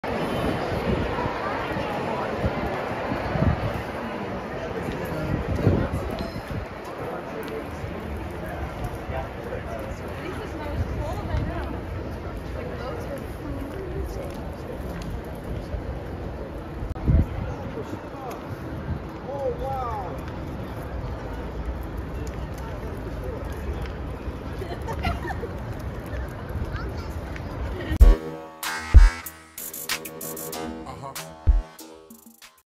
Priscilla Presley and Riley Keough arrive to the 75th Emmys in Los Angeles, CA
Priscilla Presley and her granddaughter Riley Keough look stunning as they arrive at the 75th Emmys, where she's nominated as Outstanding Lead Actress in Daisy Jones and The Six.

CREDIT: @backgrid_usa